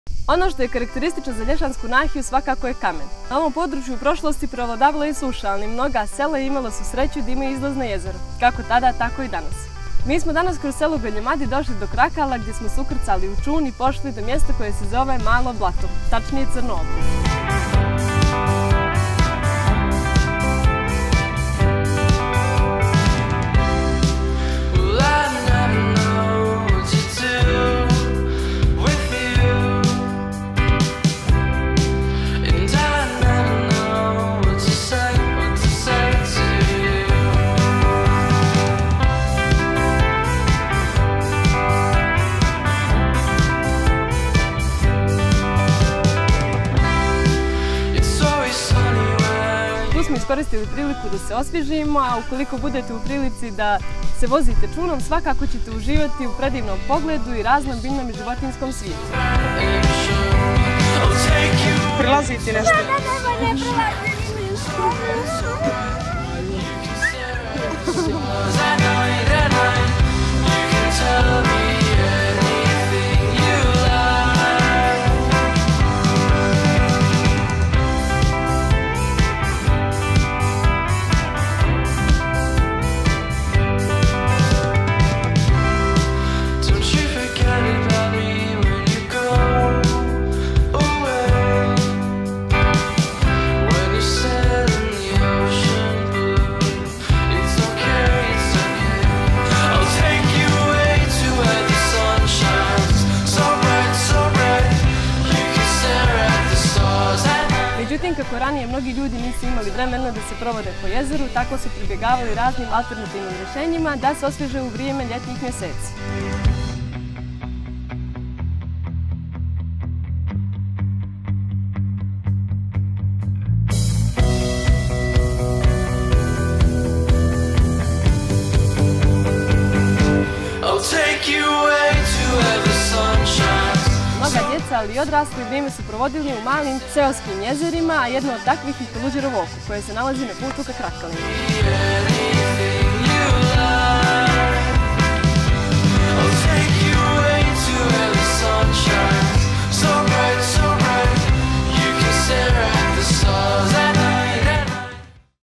LJEŠANSKA NAHIJA I SKADARSKO JEZERO, IDEALAN SPOJ U VRELIM DANIMA
Mještani i ljudi iz Lješanske nahije znaju koliko sreće imaju sela koja imaju izlaz na Skadarsko jezero. Mi vam danas otkrivamo kako izgleda Malo blato do kojeg se stiže čunom sa Krakala